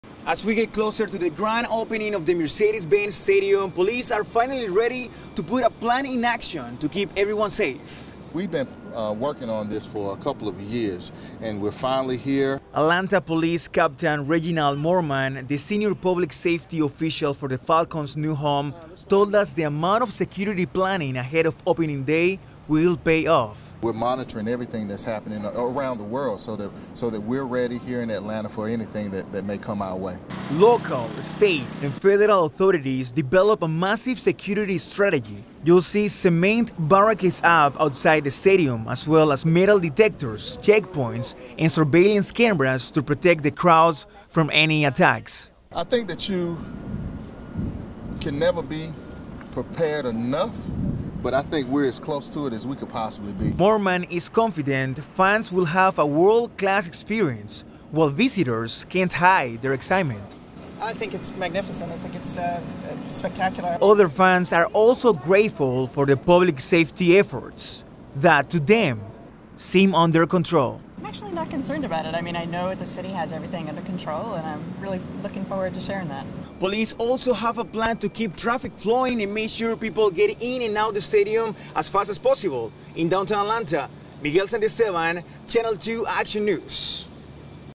Mercedes-Benz Stadium Opening Day Security
Miguel Santiesteban reports from the new MBS on the dynamics involving security planning and traffic flow prior to opening day (September Report)